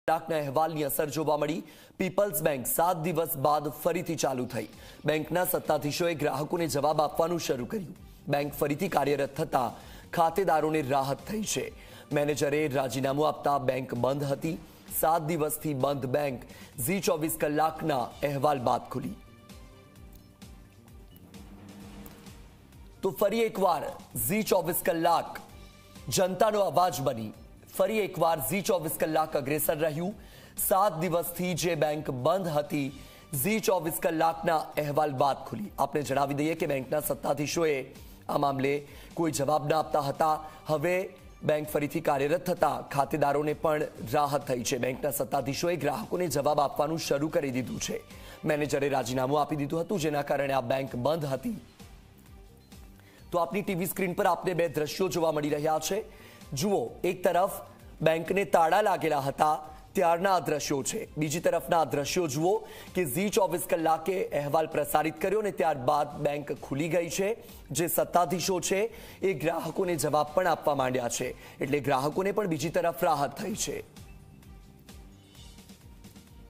Vadodara: Respite for account holders as Peoples Bank in Dabhoi reopens after being shut for 7 days
વડોદરાના ડભોઈમાં ZEE 24 કલાકના અહેવાલની અસર--

પીપલ્સ બેંક સાત દિવસ બાદ ફરીથી ચાલુ થઈ---

બેંકના સત્તાધીશોએ ગ્રાહકોને જવાબ આપવાનું શરૂ કર્યું---

બેંક ફરીથી કાર્યરત થતા ખાતેદારોને મળી રાહત--

મેનેજરે રાજીનામું આપતા બેંક હતી બંધ--

સાત દિવસથી બંધ બેંક ZEE 24 કલાકના અહેવાલ બાદ ખુલી